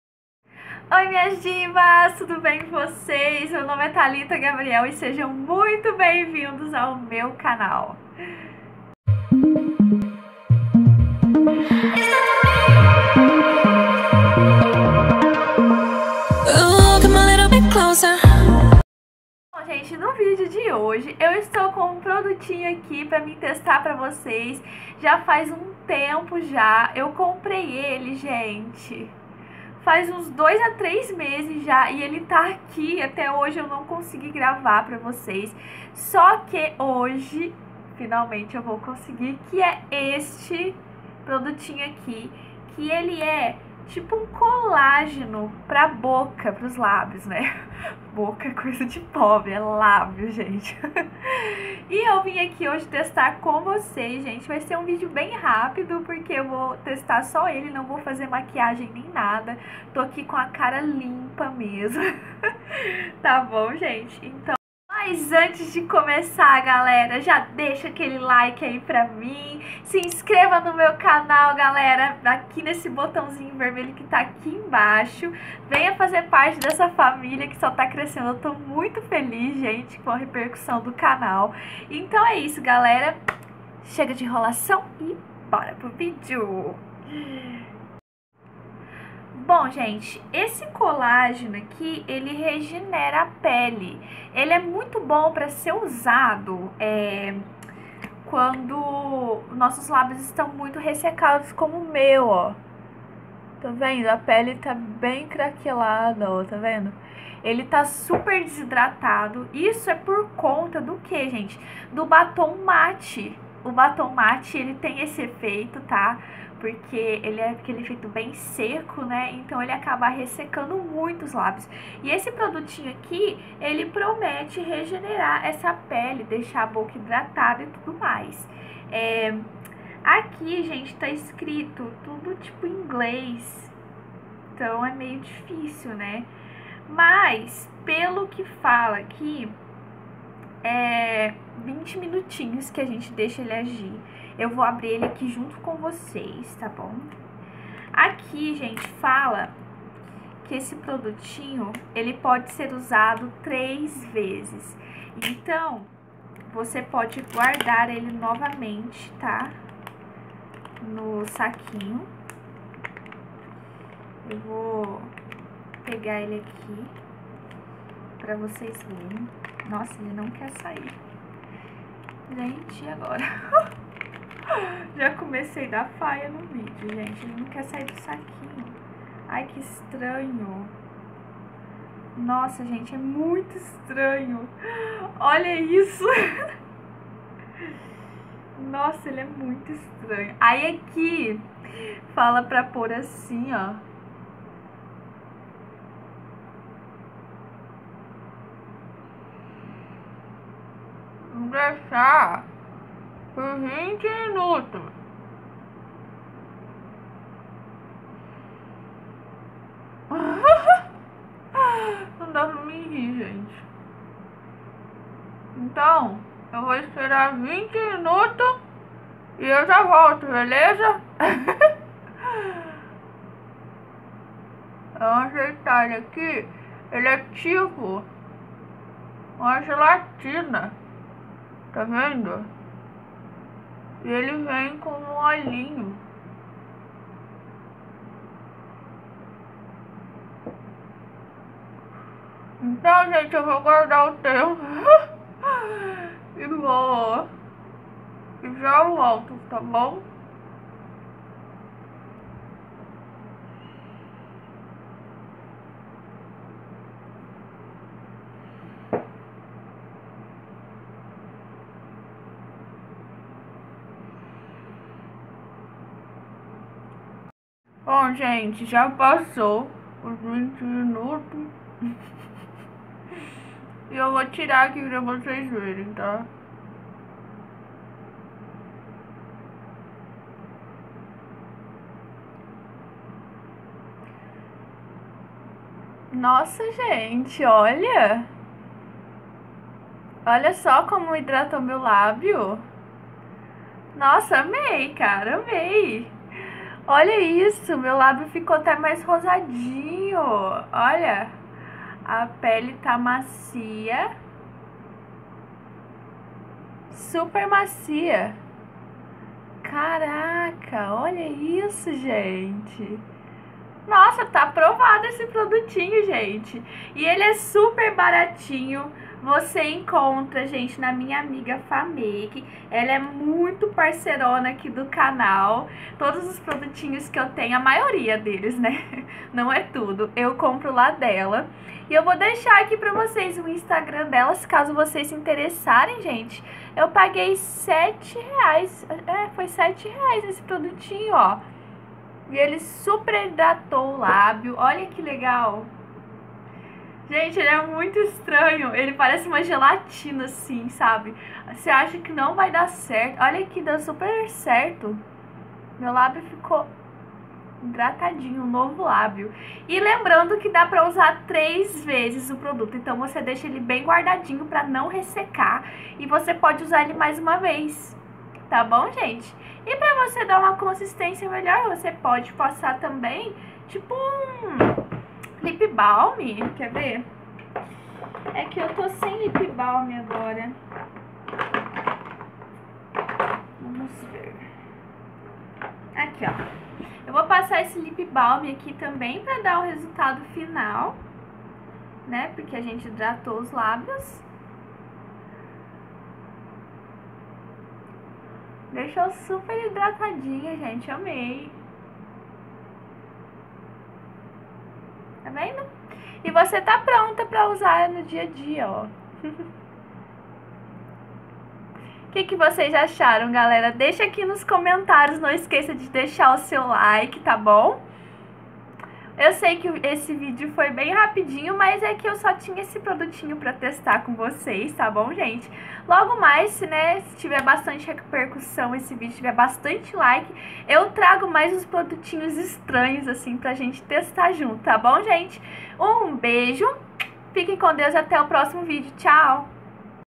Testando o Colágeno para Lábios - Funciona mesmo?
Olá meninas tudo bem? No vídeo de hoje eu trouxe pra vcs o teste de um produto maravilhoso que hidrata super bem seus lábios, espero mt q vcs gostem...
Um beijo fiquem cm Deus e até mais...

Venha vc tbm fazer parte desta família...

Se vcs quiserem adquirir este produtinho visitem a página da minha amiga Faah Make...